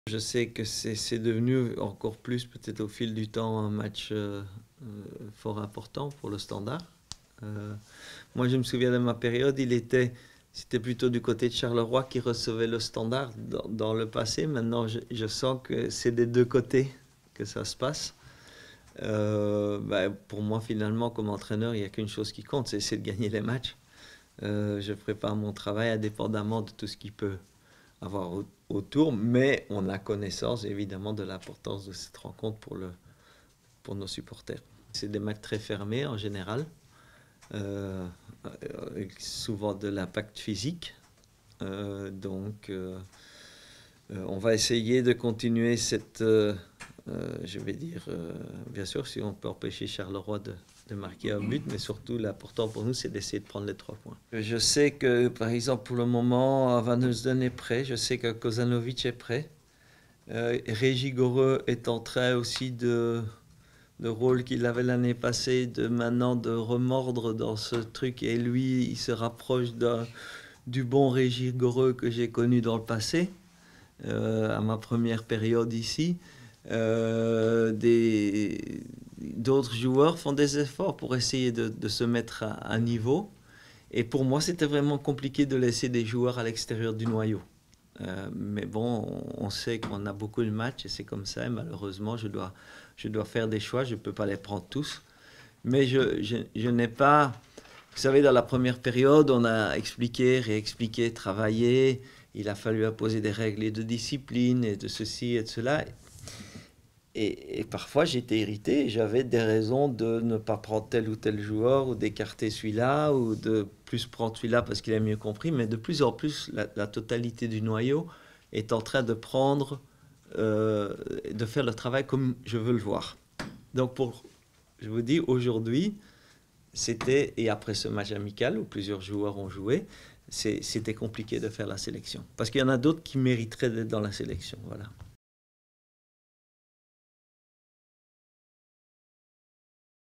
Standard - Charleroi
Conférence de presse de Michel Preud'homme avant Standard - Charleroi